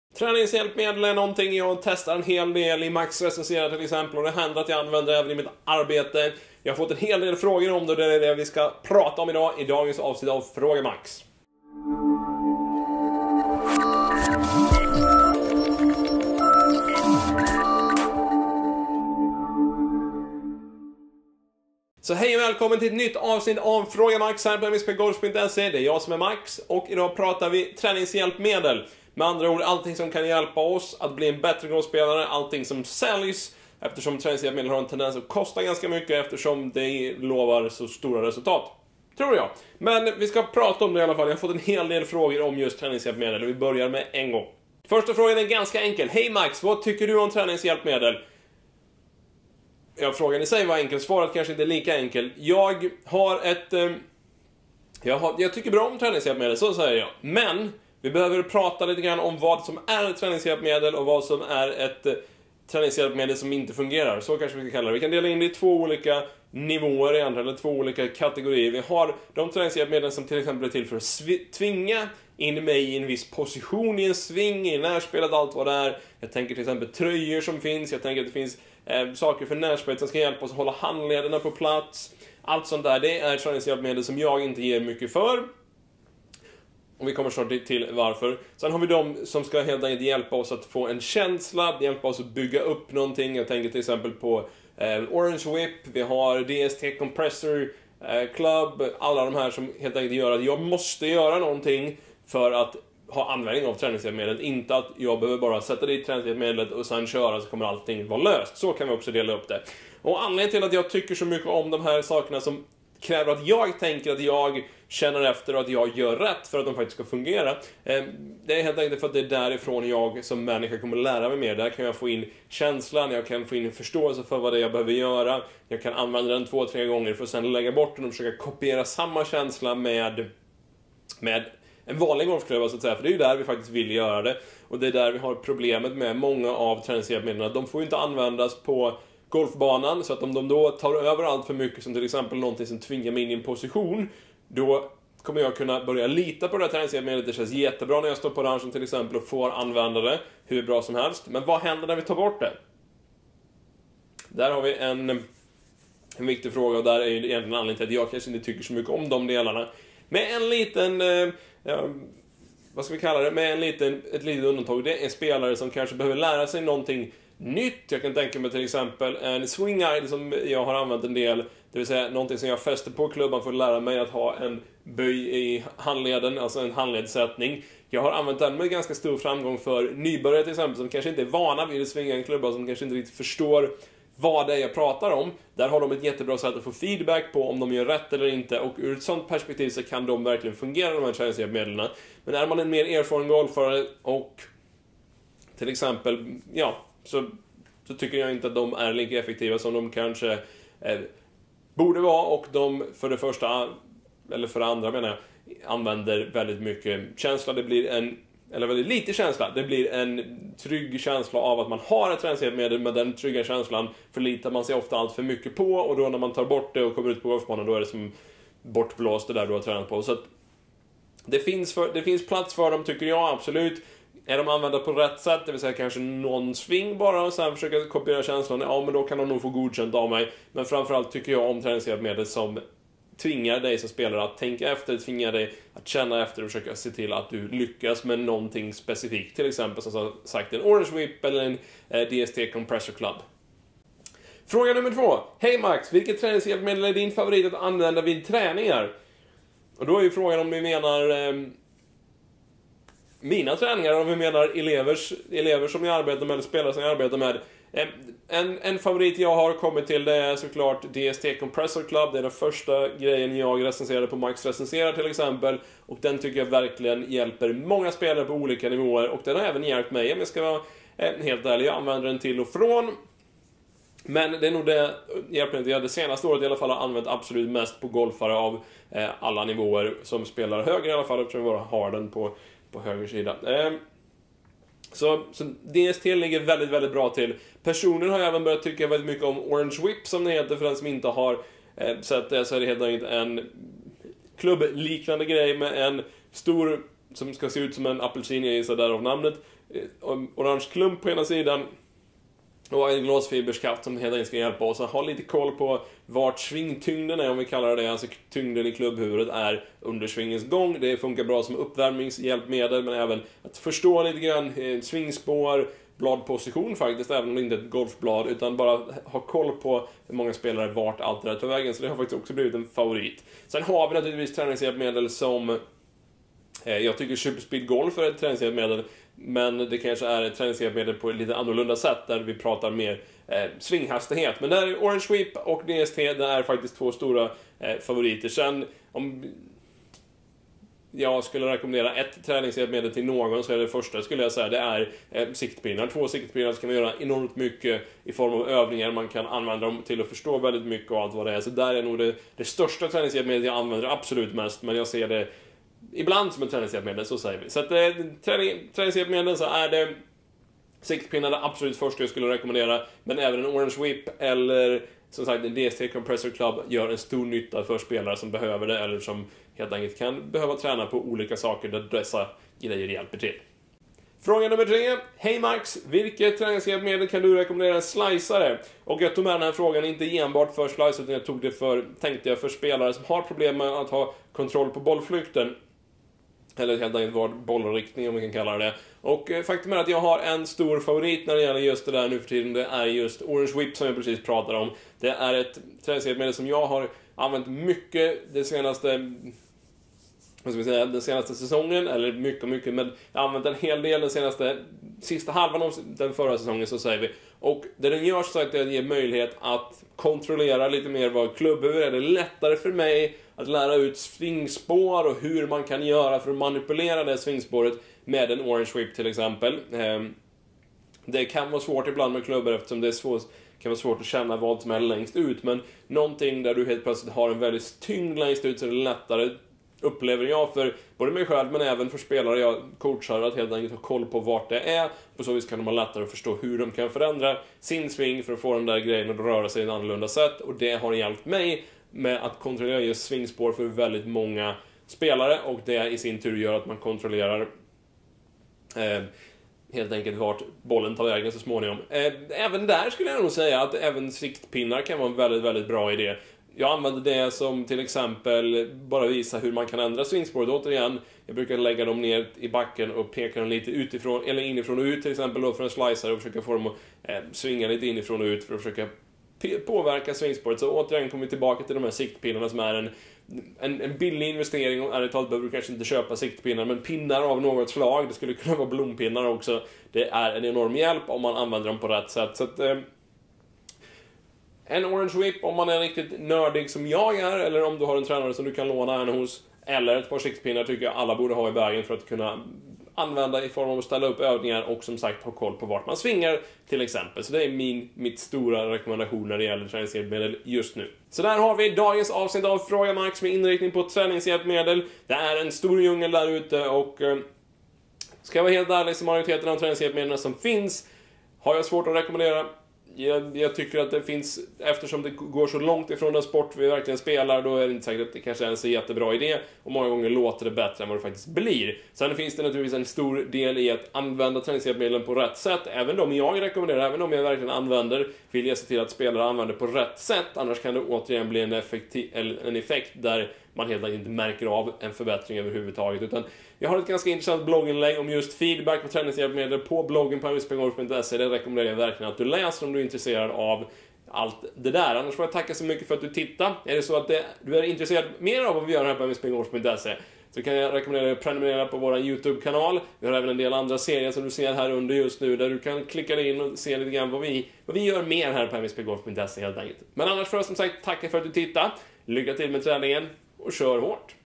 FrågaMax - Träningshjälpmedel
De finns i mängder. De säger sig ska hjälpa dig med allt och lite till. Men vad tycker en tränare faktiskt om träningshjälpmedel? Titta på dagens avsnitt av FrågaMax för att höra vad Max säger!